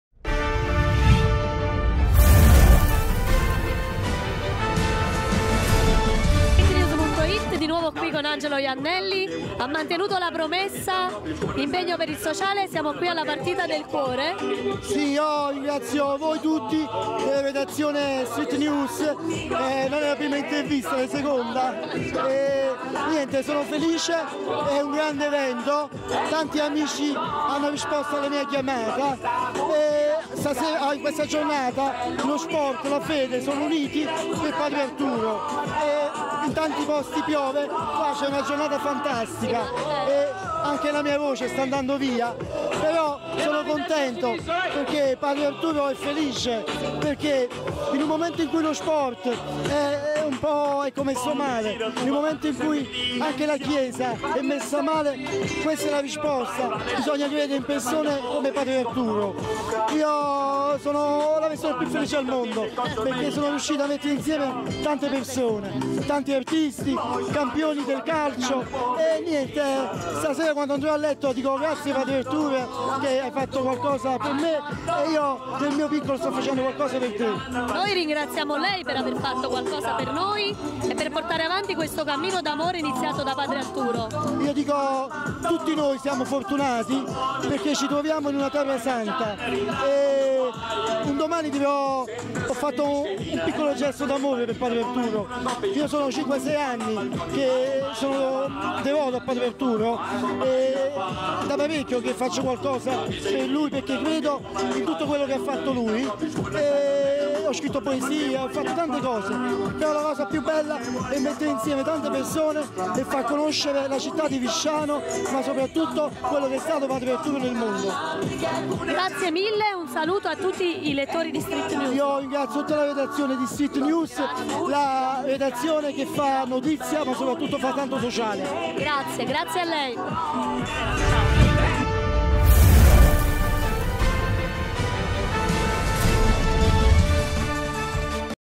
"Social Day Padre Arturo D'onofrio" Margherita Saporito intervista Angelo Iannelli
Partita del cuore a Visciano il 28/05/2015 in onore di Padre Arturo D'onofrio organizzata dall'artista Angelo Iannelli. L'inviata di StreetNews.it Margherita Saporito intervista l'artista Angelo Iannelli.